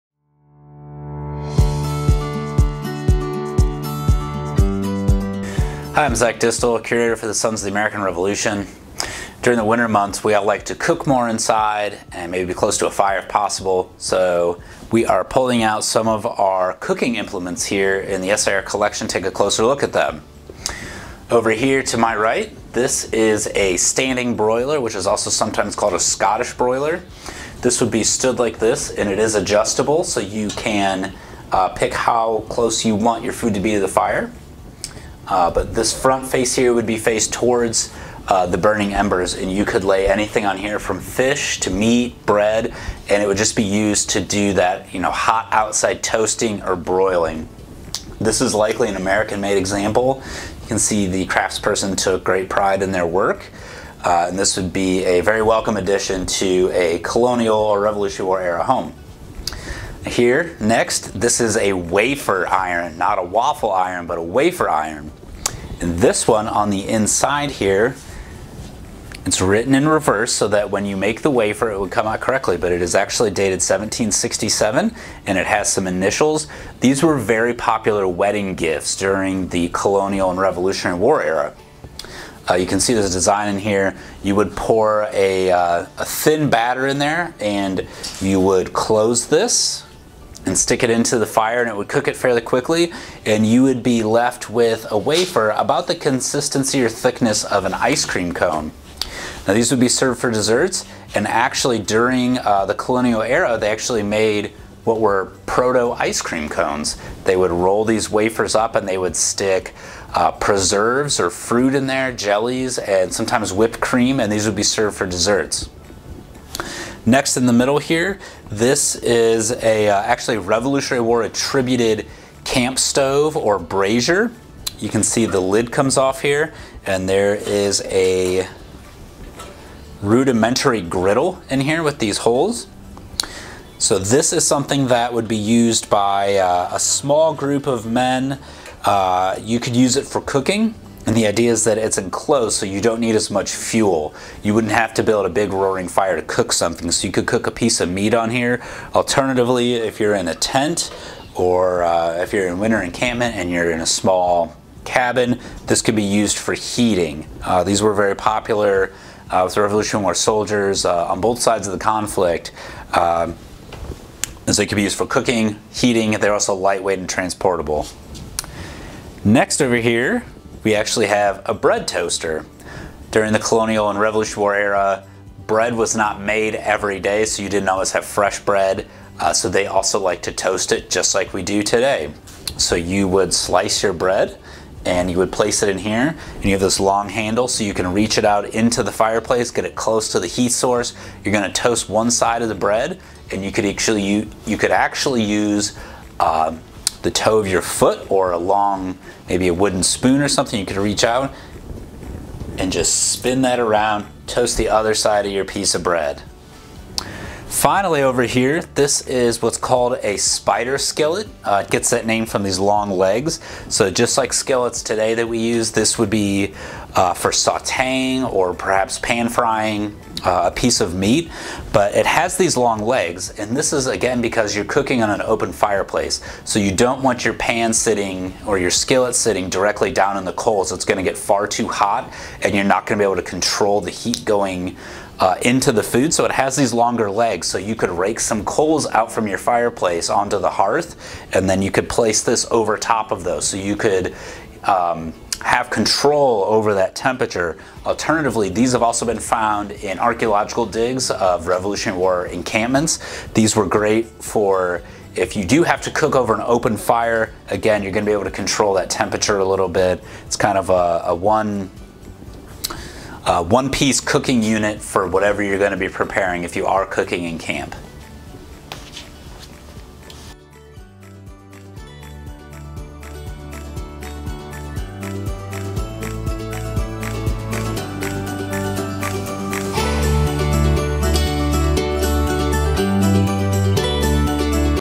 Selections from the Collection: Cooking Implements
Zac Distel, Curator for the Sons of the American Revolution describes various cooking implements that were used during the 18th century!